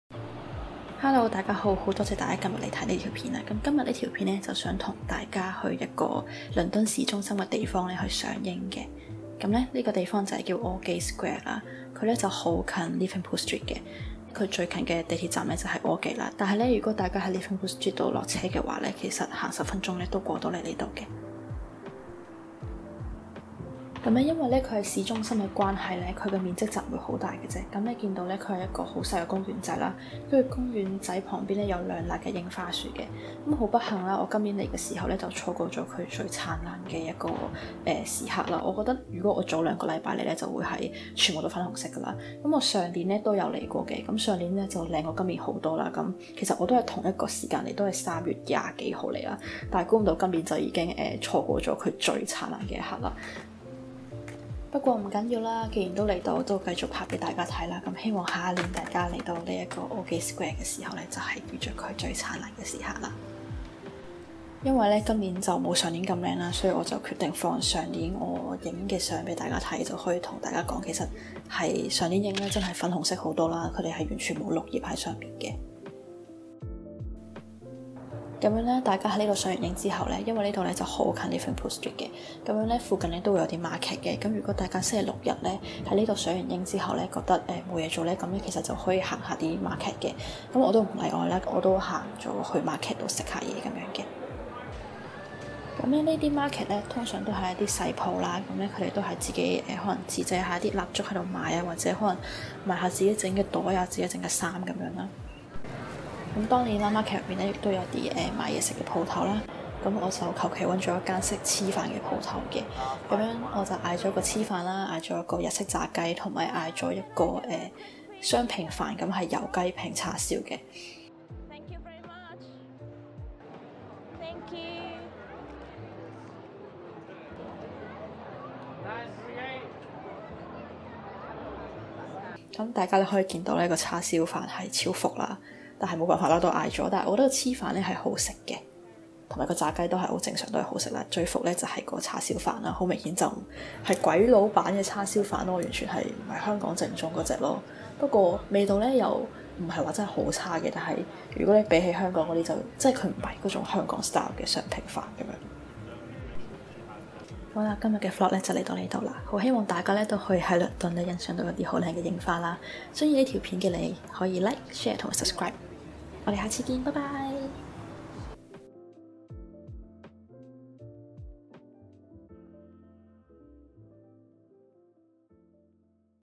【 倫敦櫻花🇬🇧】Aldgate Square 賞櫻日 🌸| Spitalfields Market 皮塔佛德市集
TIMESTAMPS

→ 0:00​ - Aldgate Square
地址：Aldgate High St, London EC3N 1AF

→ 01:22 - Spitalfields Market  皮塔佛德市集
地址：56 Brushfield St, London E1 6AA